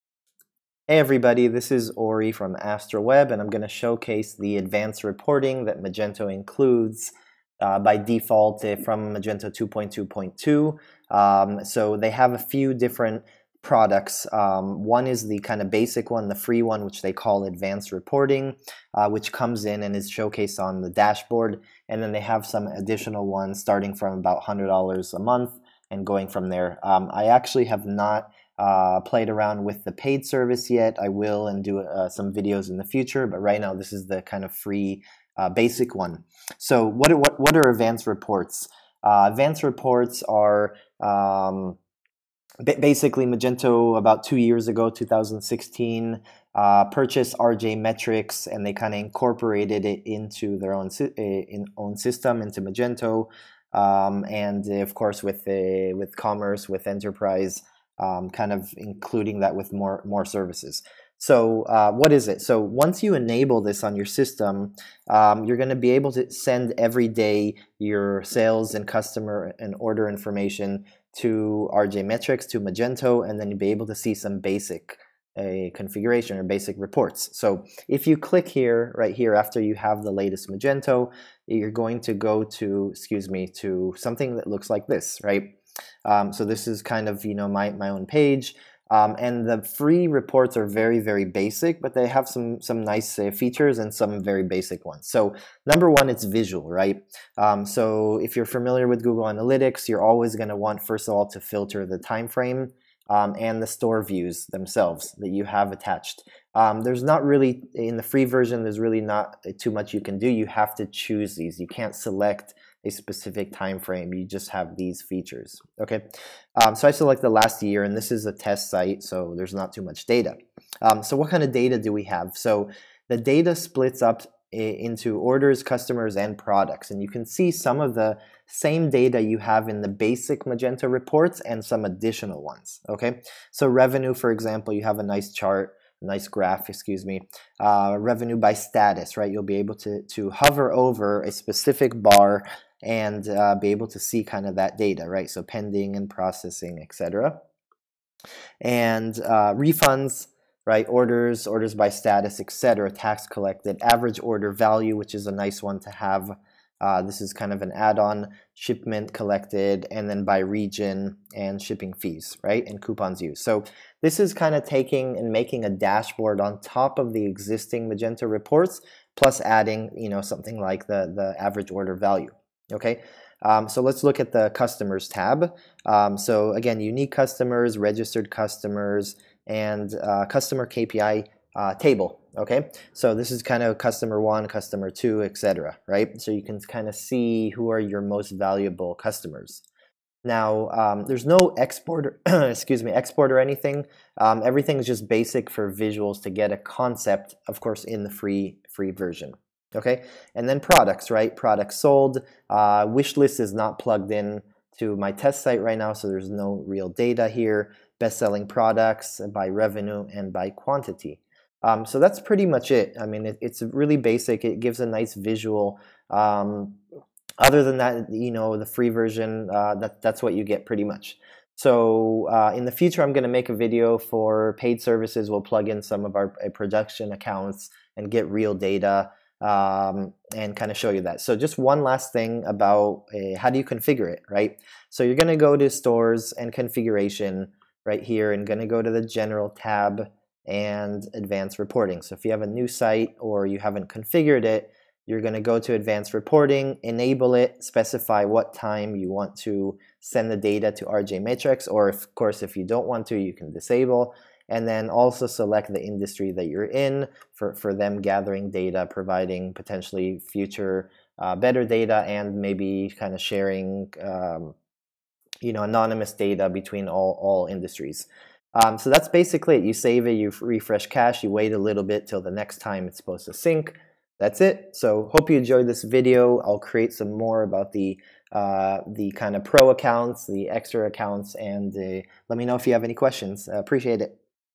Magento 2 Advanced Reports (BI)
Showcasing the free advanced reports that comes with Magento 2.2.2 or later which includes Rj Metrics (BI) platform purchased by Magento.